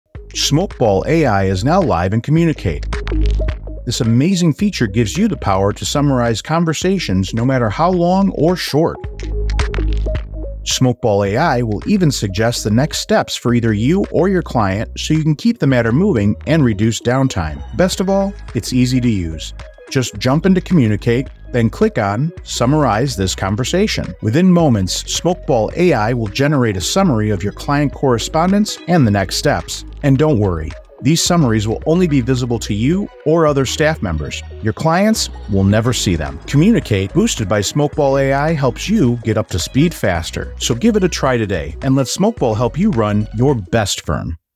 Summarize conversations in a click with Smokeball AI
Smokeball AI produces a quick summary of your client conversation and lists next steps. It saves you time trying to reconstruct the conversation so you can get to helping your clients.

✨ Just one of many ways Smokeball AI is boosting productivity! ✨